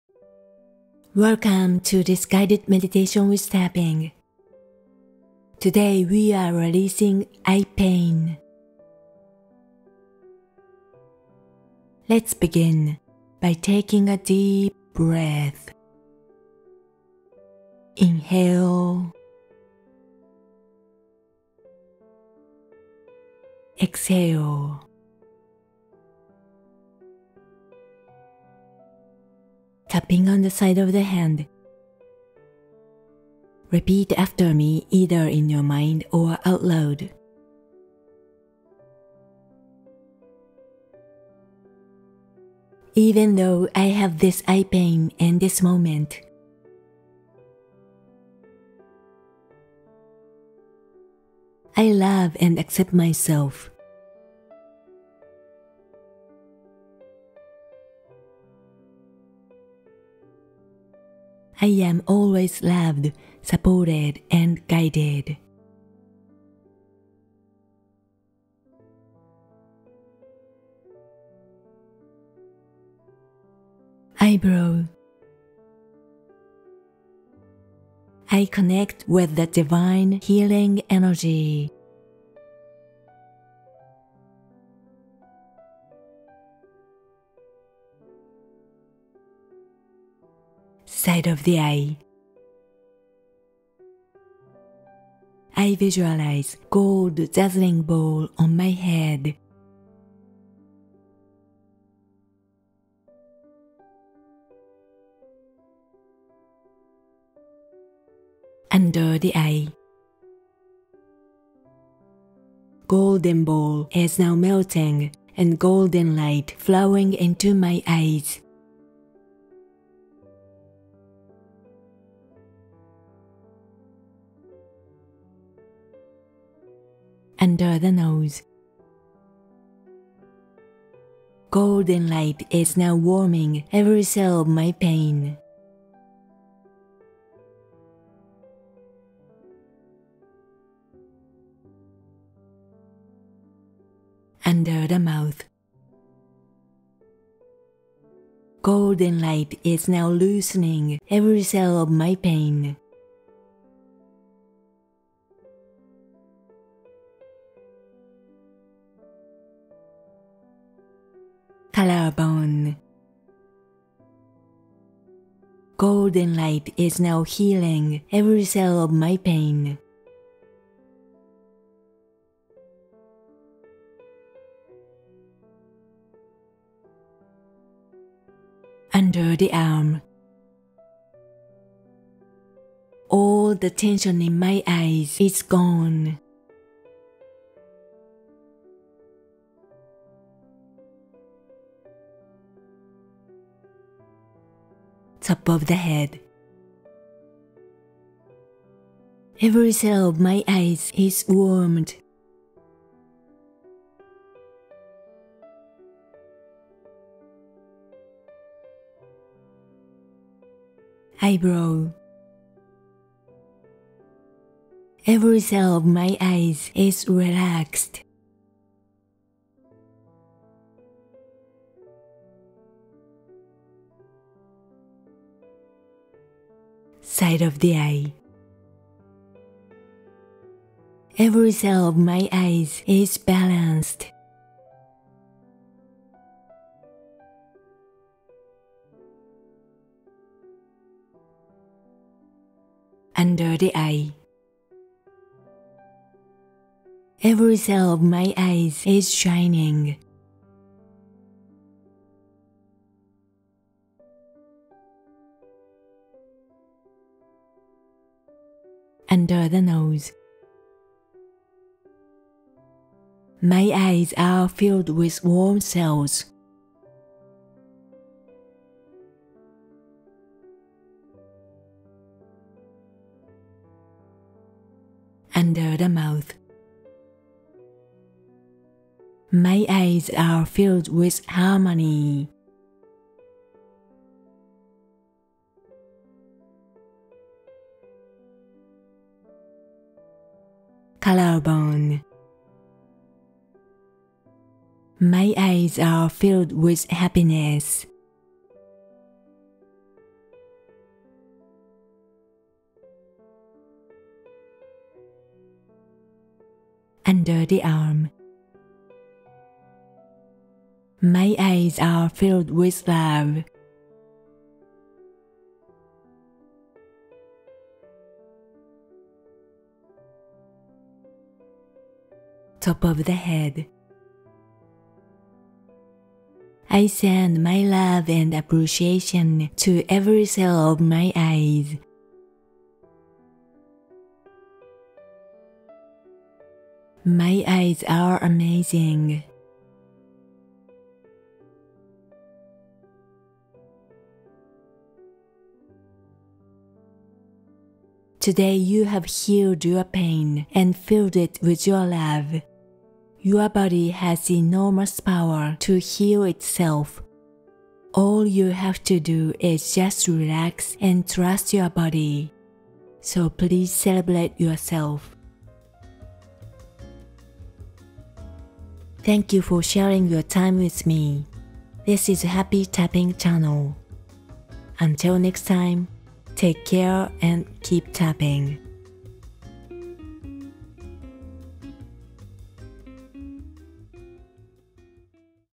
[5min] Quick Tap Into Eye Pain With EFT Tapping -- Simple & Powerful Tool To Heal Your Body -- 171
00:00  Intro
00:11  Deep Breath
00:31  Setup Statements (Focus on your Stress)
01:13  Meditation with Tapping
05:47  Ending

EFT Tapping is strongly effective to eliminate negative energy from your body.

While Tapping sends a quick signal to your brain to lower the cortisol level (known as a stress hormone), Tapping and Visualization enhance your body's power to restore and help you release the pain in your body. Please enjoy this process and give yourself a gift of a healthy, joyful life.

Tapping and Meditation is a simple but really powerful and effective combination so you can release your stress or tension instantly, and find yourself in a peaceful state of mind in just a few minutes, even in seconds. (Somehow) it really works!!

I’ll guide you through this process and tap along, so please join me and let’s Tap together!

If your SUD level is still higher than you'd like it to be, please feel free to repeat the process daily.

【Camera】SONY Alpha ZV-E10
【Lens】SONY SEL35F18